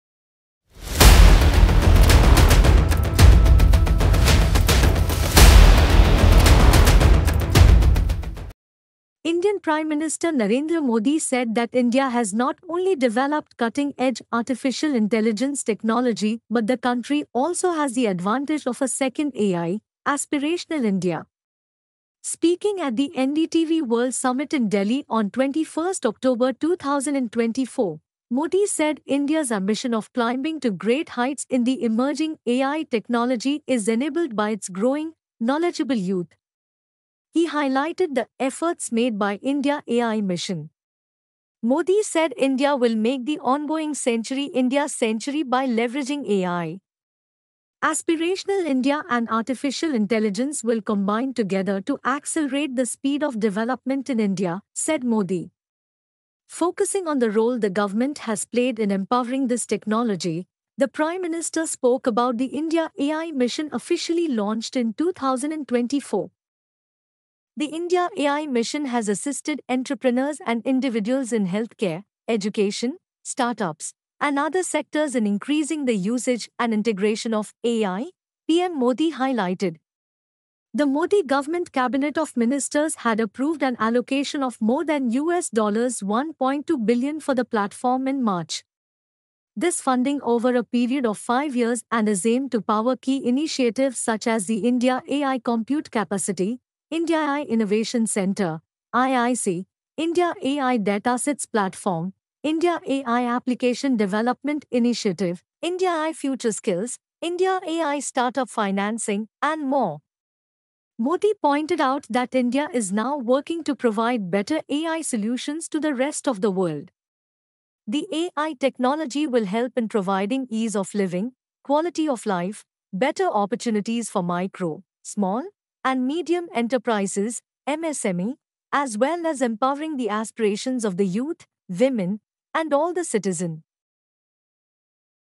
Modi says India’s AI will transform country, world
Indian Prime Minister Narendra Modi said that India has not only developed cutting edge Artificial Intelligence technology but the country also has the advantage of a second AI — aspirational India. Speaking at the NDTV World Summit in Delhi on 21st October 2024, Modi said India’s ambition of climbing to great heights in the emerging AI technology is enabled by its growing, knowledgable youth. He highlighted the ‘Efforts Made by India AI Mission.’
Modi said India will make the ongoing century India's century by leveraging AI.